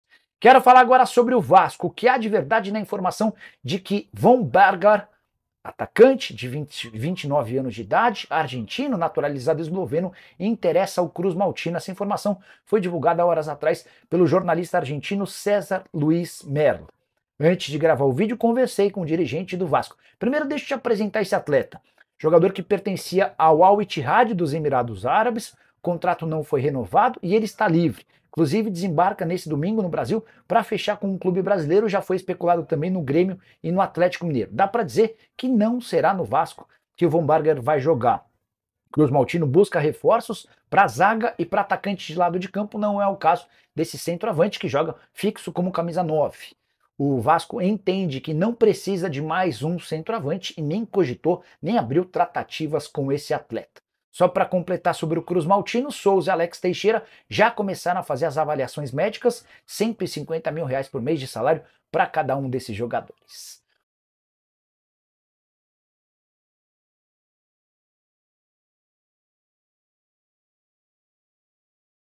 VASCO FAZ PROPOSTA PELO ATACANTE ANDRÉS VOMBERGAR
Bem-vindo ao canal de Cortes do Nicola! Fala, galera! NESTE CORTE, vamos falar sobre a proposta do Vasco pelo atacante Andrés Vombergar. Vamos discutir os detalhes da negociação e o impacto dessa possível contratação para o time. Não perca!